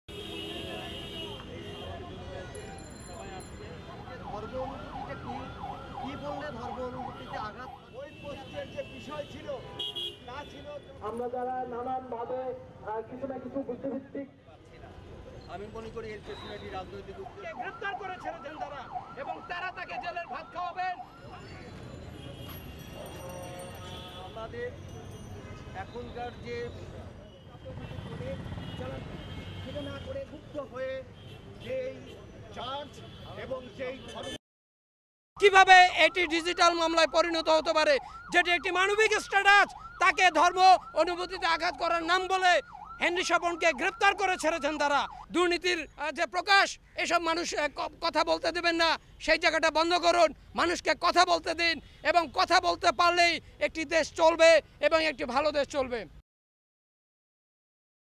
কবি হেনরী স্বপনের নিশর্ত মুক্তির দাবি
কবি হেনরী স্বপনের নিশর্ত মুক্তি ও দমনমূলক আইন বাতিলের দাবিতে 'নিপীড়নের বিরুদ্ধে শাহবাগ' সংগঠন বিক্ষোভ সমাবেশ করে।

বিকেলে বিক্ষোভ সমাবেশে বিভিন্ন প্রকাশনীর প্রকাশক ও লেখক একত্রিত হয়ে ডডডজটাল নিরাপত্তা আইনে হেনরী স্বপনের বিরুদ্ধে মামলায় করায় তীব্র নিন্দা ও ক্ষোভ প্রকাশ করেন। সমাবেশে বক্তারা